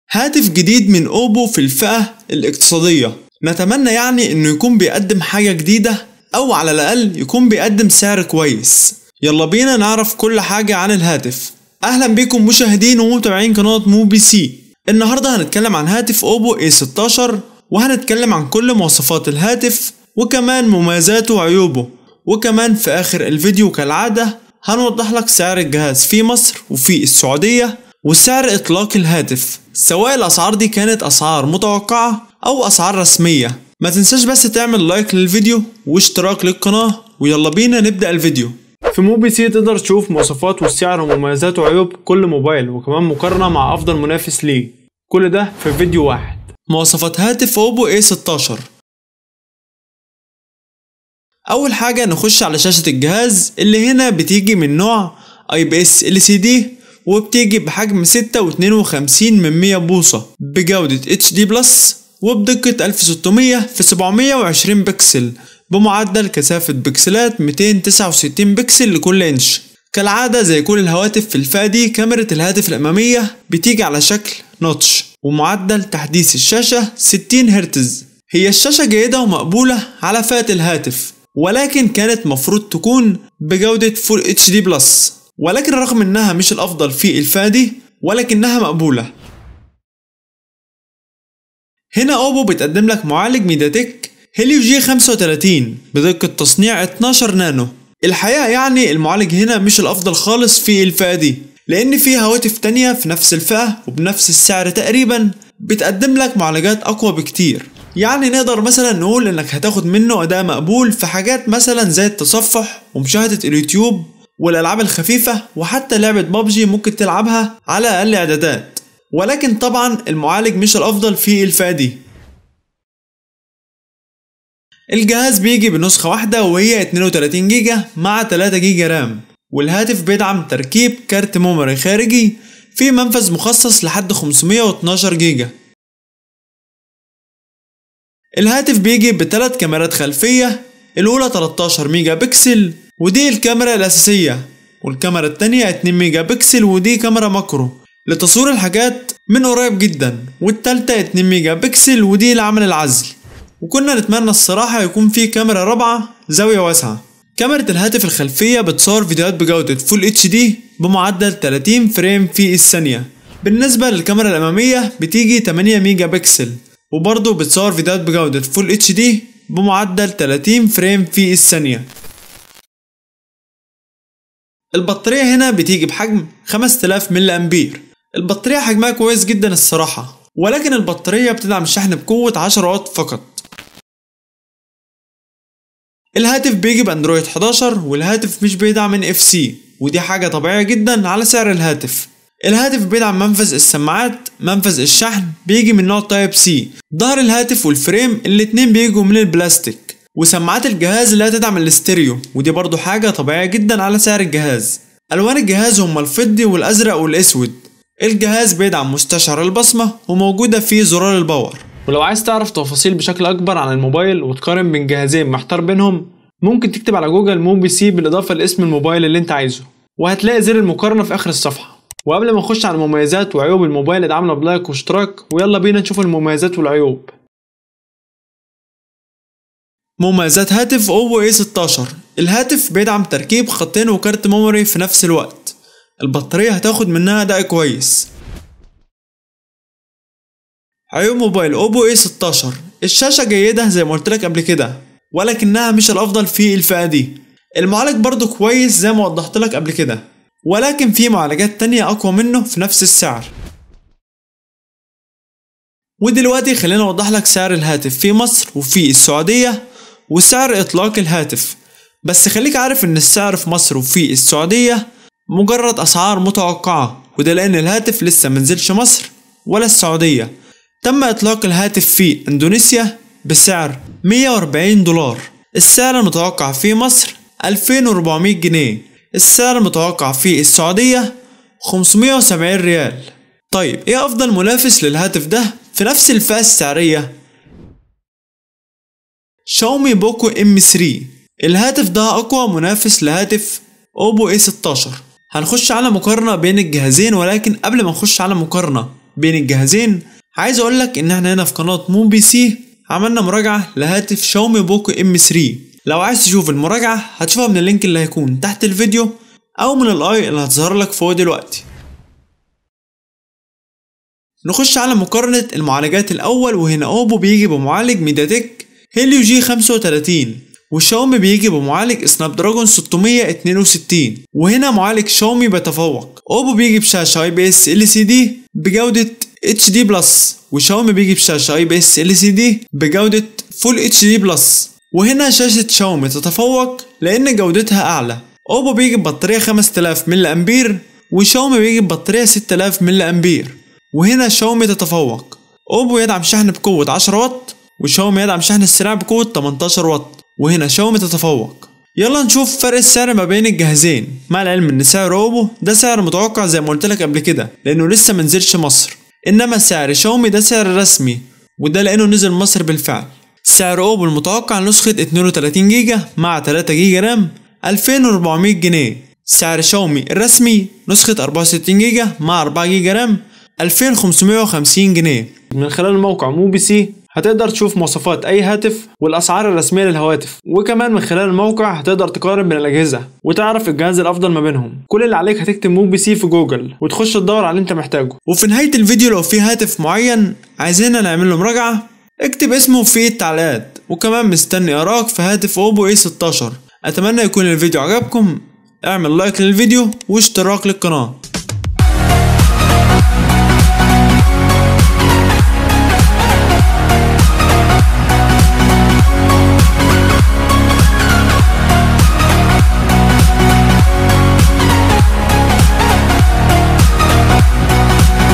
مراجعة Oppo A16 .. (نفس الغلطات ؟!)
Oppo A16, هاتف جديد من شركة اوبو موجة للفئة الاقتصادية, بس هل ممكن مثلا اوبو تقدم حاجة جديدة وسعر كويس ؟!

علشان كدة احنا انهاردة بنعمل مراجعة Oppo A16, وكمان هنوضحلك سعر Oppo A16 في مصر وفي السعودية وسعر اطلاق الهاتف في الخارج.

بجانب اننا هنقول مواصفات Oppo A16, برضو هنقول مميزات وعيوب Oppo A16.

طبعا احنا في الفيديو مش بنقول مواصفات Oppo A16 بس, لا وكمان عملنا مقارنة بين هاتف Oppo A16 وهاتف Xiaomi Poco M3 الي هو موجود في نفس الفئة.

موبي سي